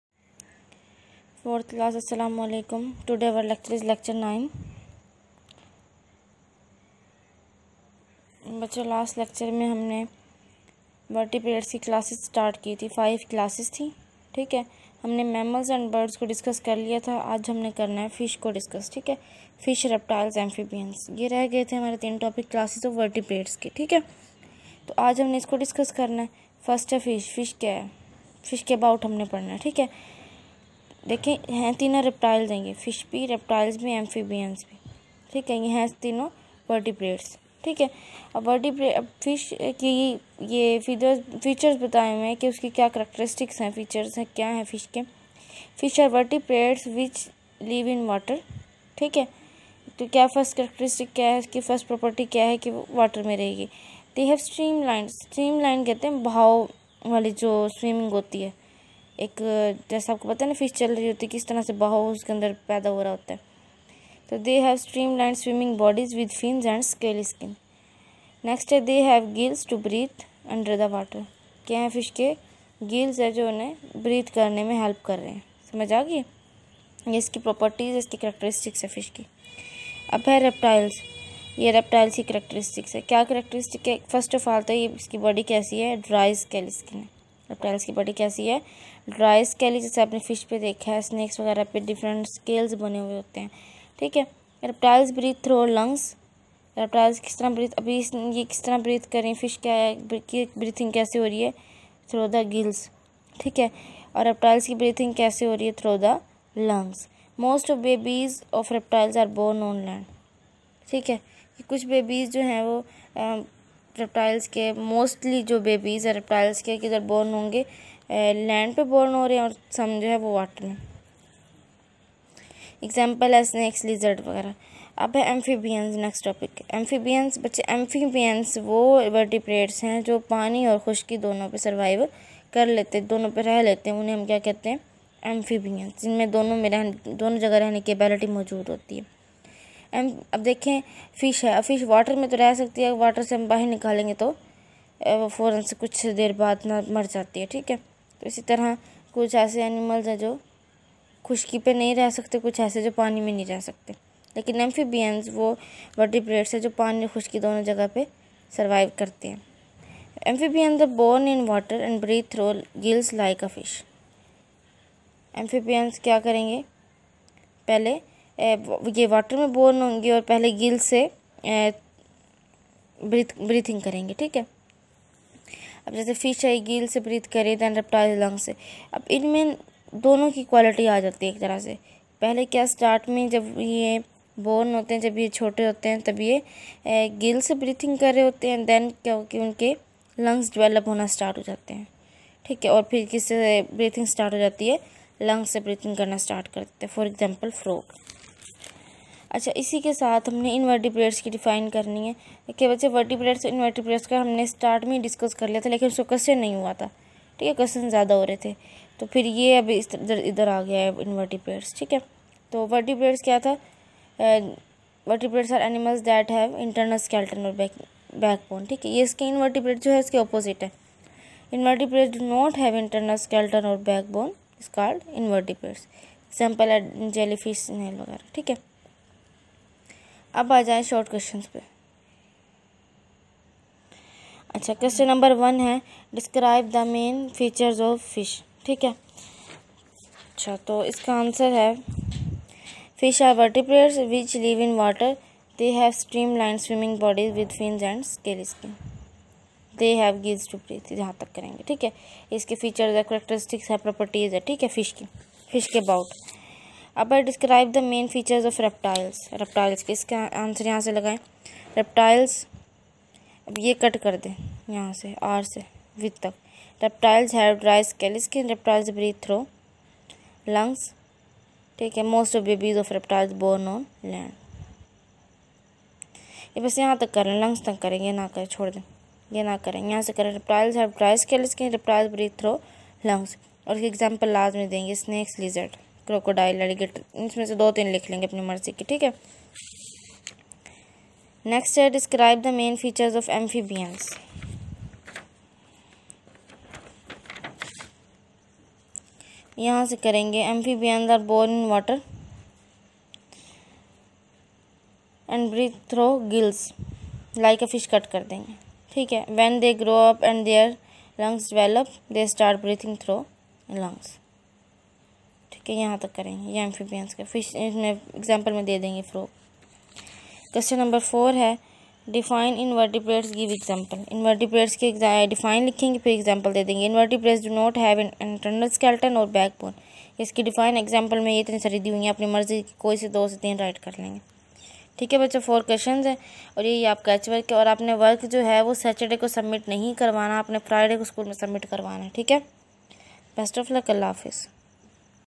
lecture 09 science 4th (06-04-2021)
remaining classes of vertebrates